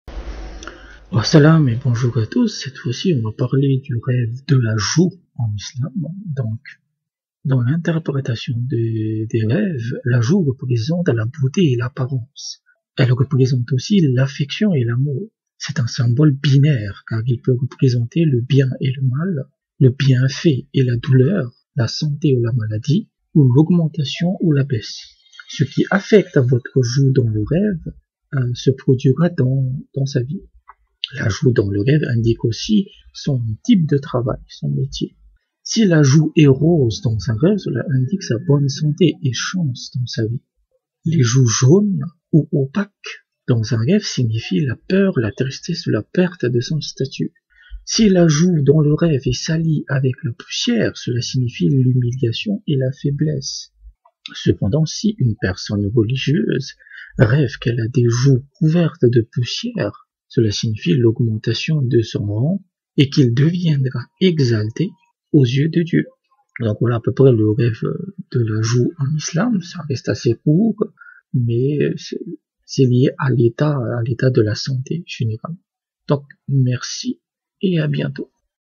Rêver de la joue en Islam
Qu'est-ce que cela signifie quand on rêve de la joue en Islam.

La plupart des photos proviennent de sites comme Pixabay ou Unsplash.